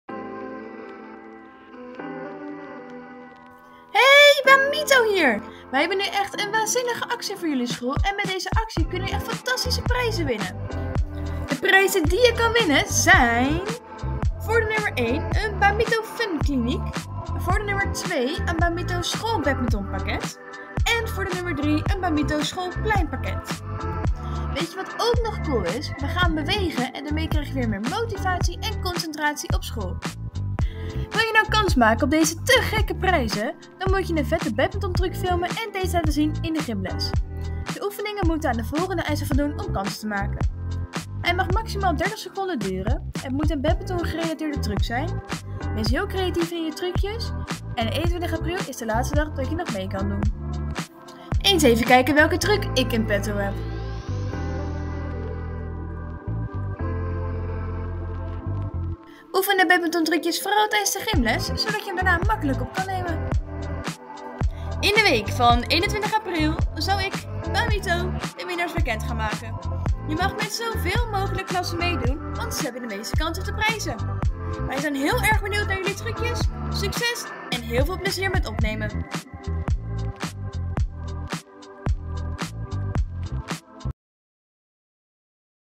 Doe mee met jouw school en win!
Verzin een leuke challenge net als Bamito en maak kans op mooie prijzen voor jouw school.